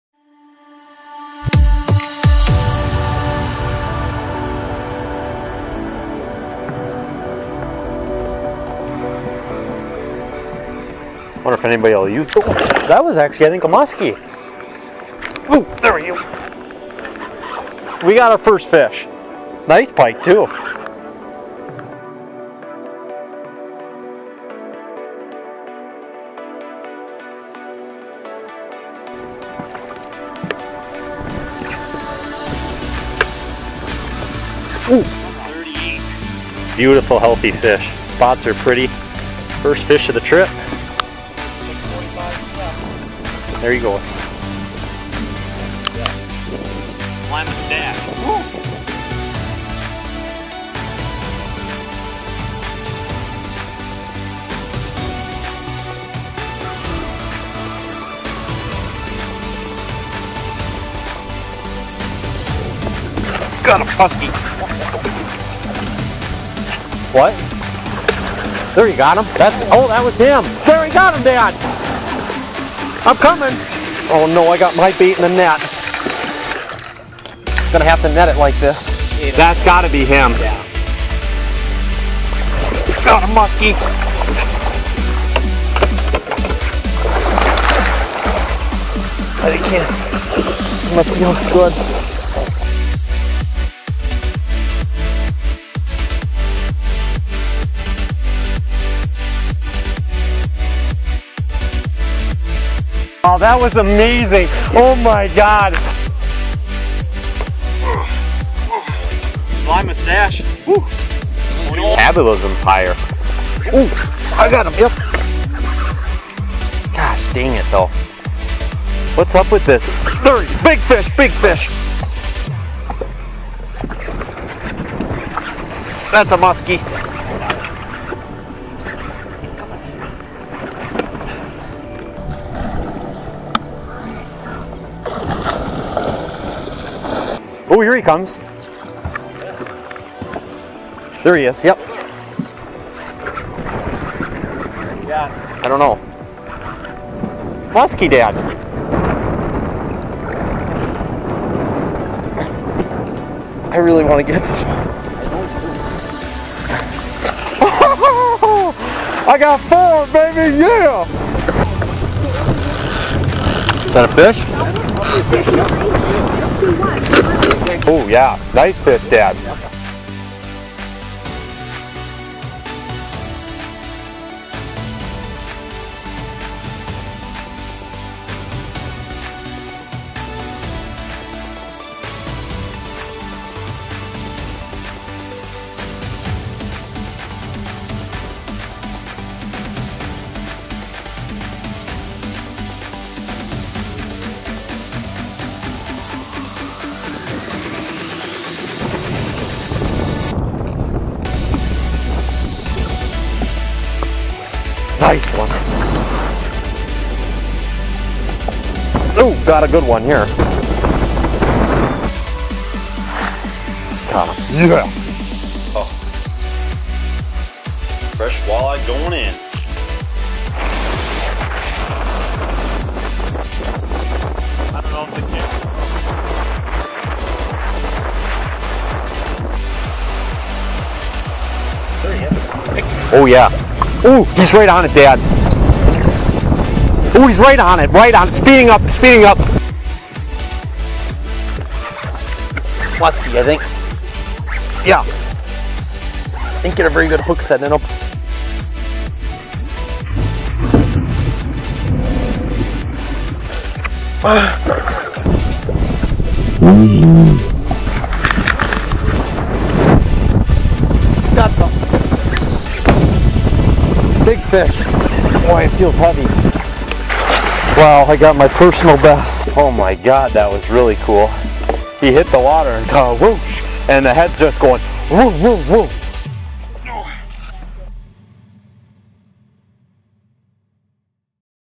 Canada Musky Trailer (2019)
This is a trailer to my week long Canada trip!!! Action packed full of great adventure and big Muskies with some amazing strikes. Can't wait for opener and to get back after these slimy creatures! Enjoy!